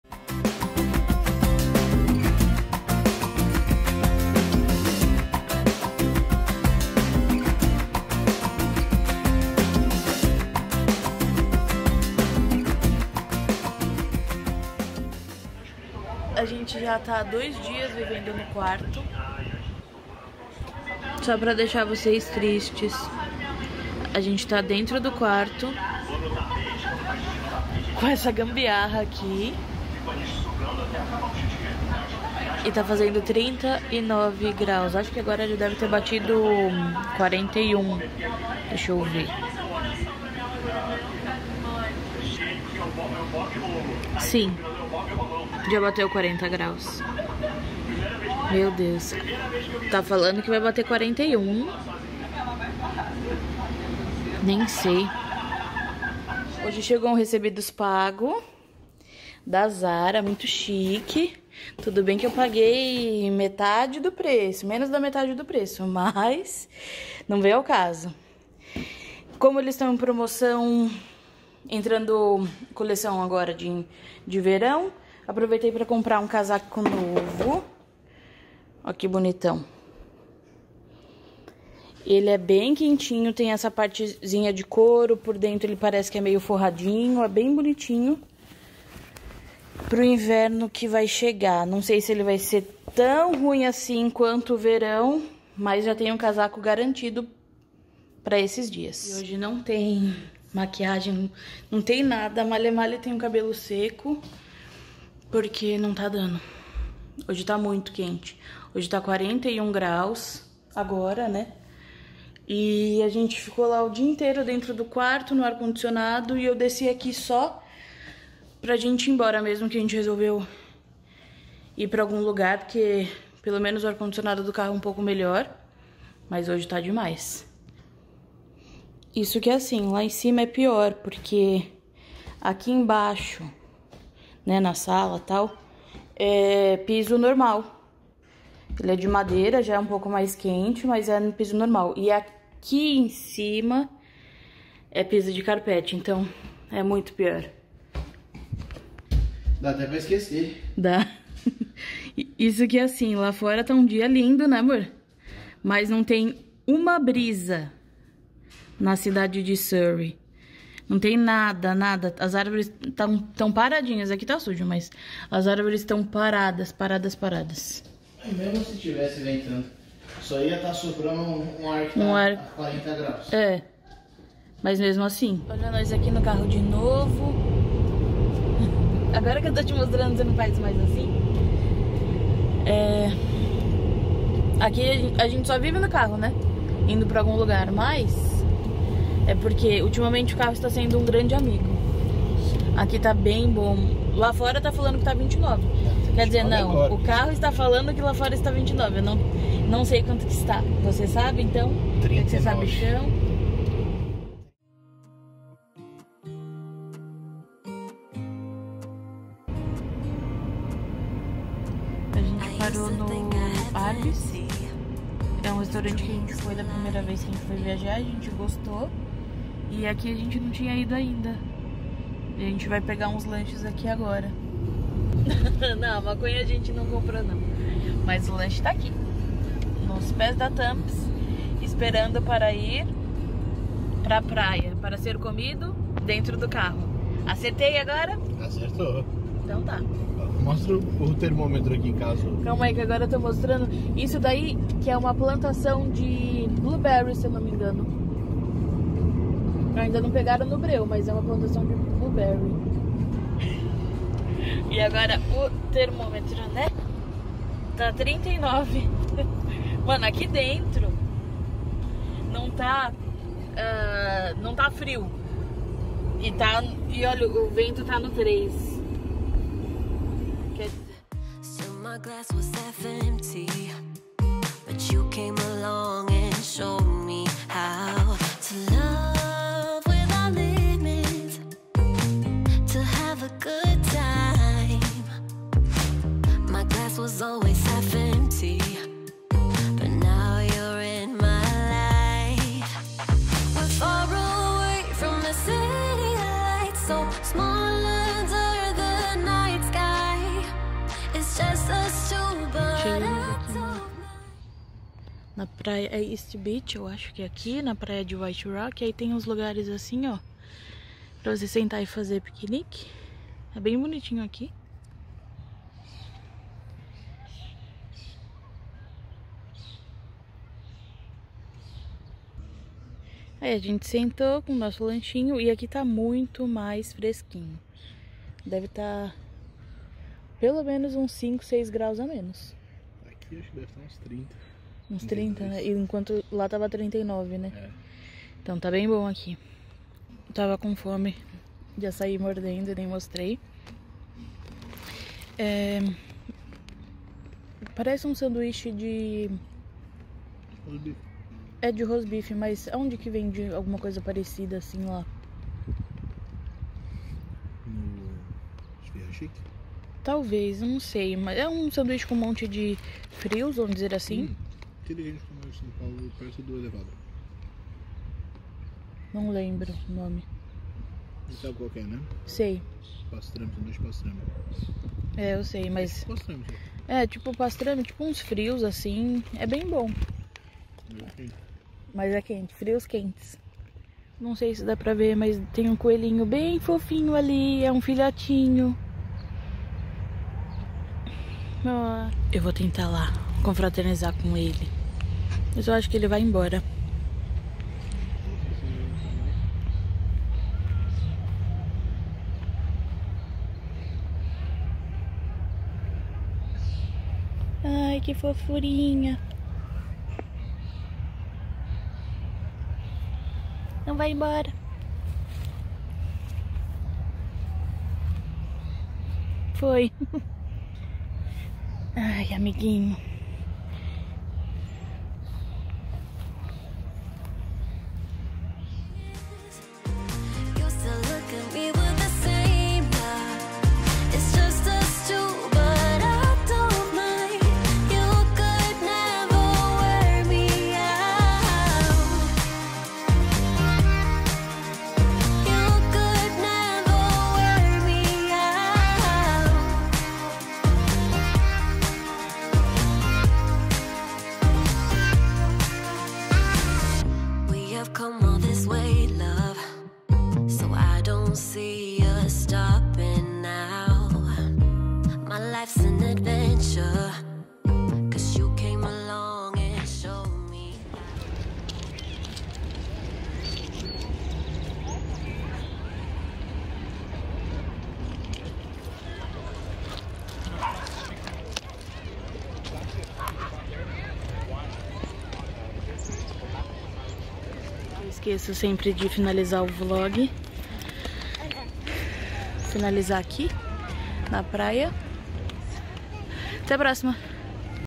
WHITE ROCK, ARBY`S NO CANADA E O DIA EM QUE VANCOUVER FERVEU! - ROLÊ DE BOTAS EP 03
Pensa num dia quente. Agora multiplica por 10!
Gente, tava muito quente!!! Tão quente que você não conseguia respirar!! 🥵